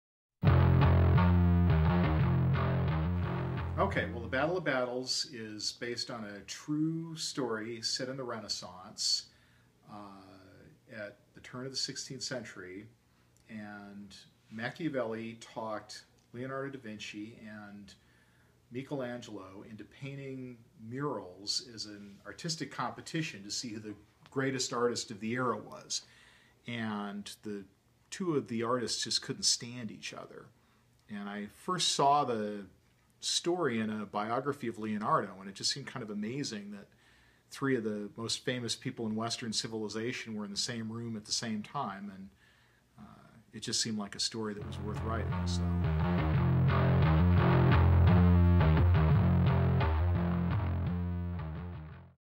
Shelterbelt Theater; The Battle of Battles
Playwright Joseph Basque introduces 'The Battle of Battles' running April 17th through May 11th at Shelterbelt Theater.